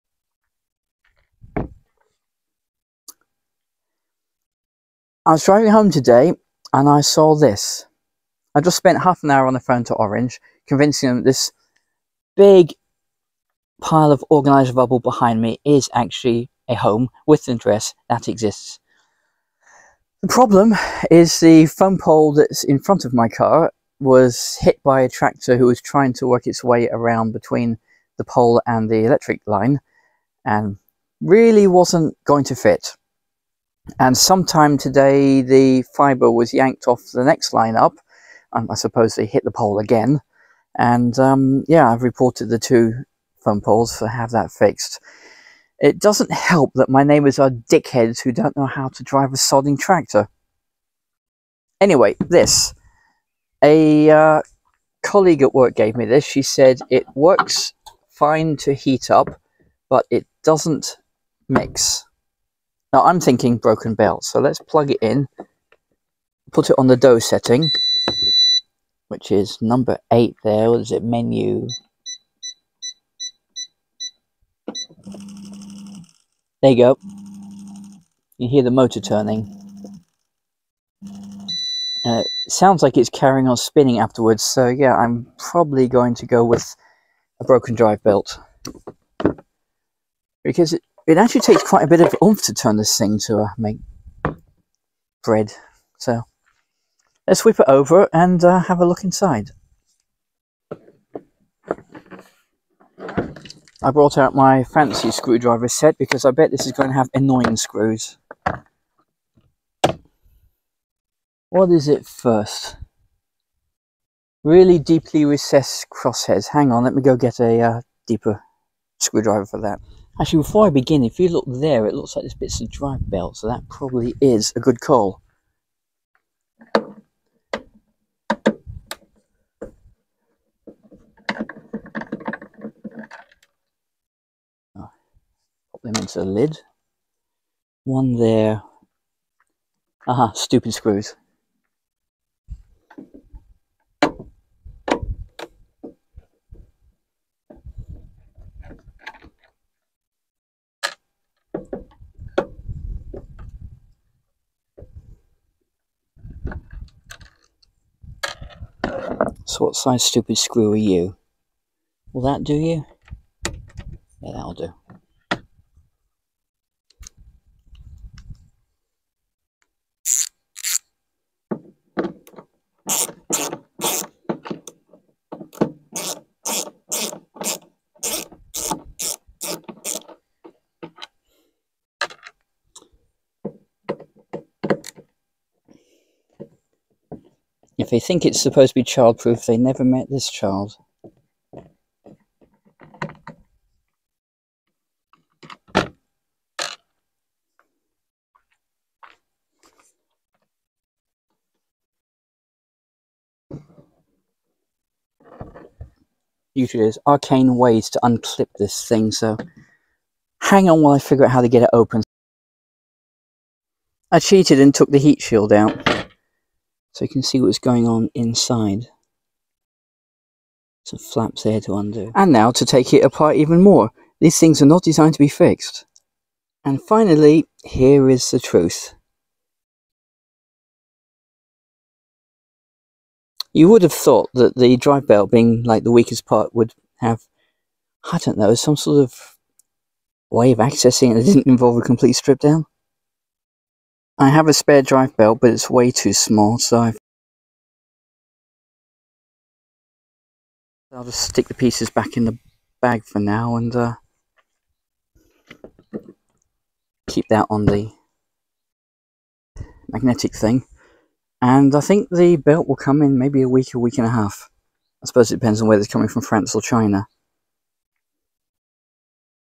A broken bread maker (part one)
A work colleague gave me an old bread maker. She says it heats, but won't knead the dough.

She has a new one she got from a four-letter German place, but she isn't at all happy with it as the bread never rises properly.

So my plan is to fix this one, obviously very well used (it has made a LOT of brioche, apparently!) and give it back to her.